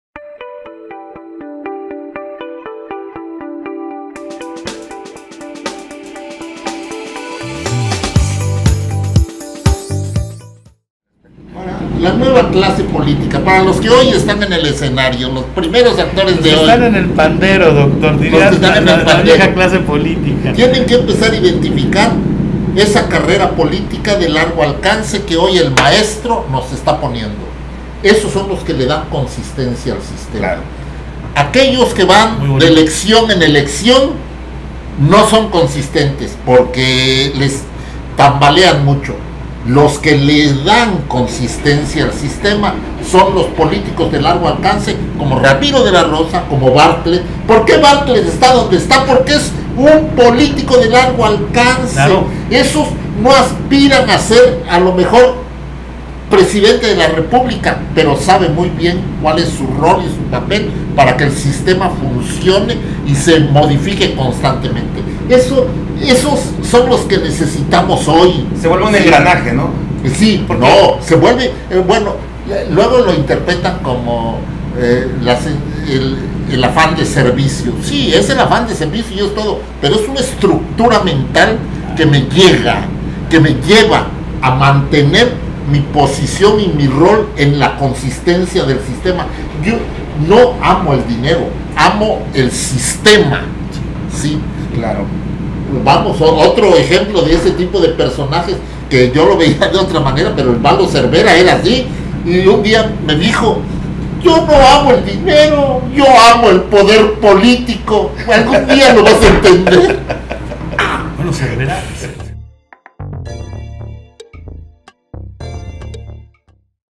“LOS POLÍTICOS DE LARGO ALCANCE LE DAN CONSISTENCIA A LA POLÍTICA”
*Saben su rol y su papel para que el sistema funcione; no aman el dinero, aman el poder político, considera el Dr. Tomás Contreras Castillo.
(Extracto 6 de RelatosDeDonTomás con Jonathan Estrada vs. Jorge Rodríguez Carrillo)